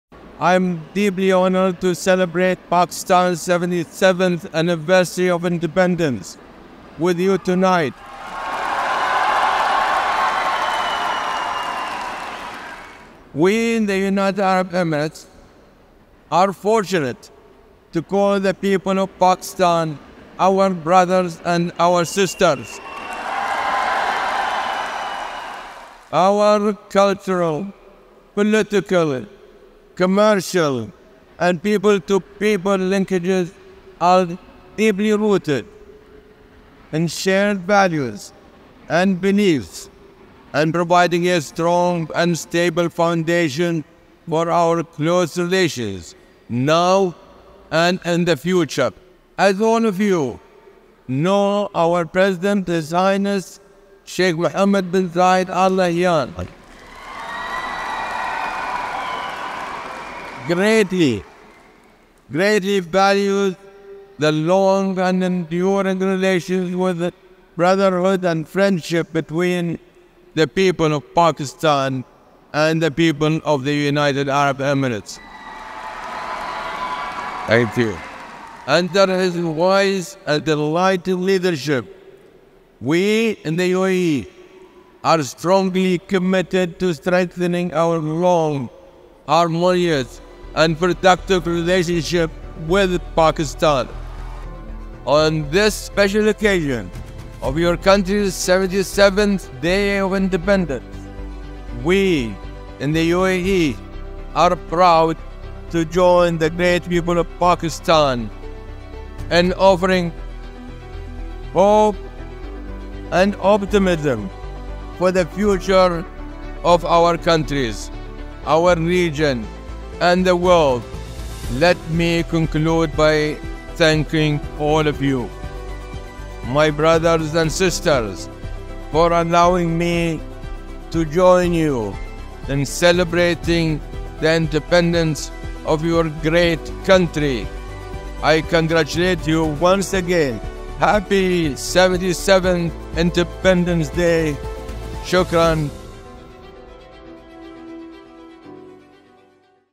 Bank Alfalah | Independence Day | H.E Sheikh Nahayan Mabarak Al Nahayan
On the joyous occasion of Pakistan's 77th Independence Day,  H.E Sheikh Nahayan Mabarak Al Nahayan, Minister of Tolerance & Coexistence and Chairman Bank Alfalah, extends his heartfelt wishes to the people of Pakistan.

May this day inspire unity, prosperity, and continued progress. Here's to celebrating the enduring bond between our nations and a future filled with peace and harmony.

Customers may register their complaint through SBP’s customer complaint management service ‘Sunwai’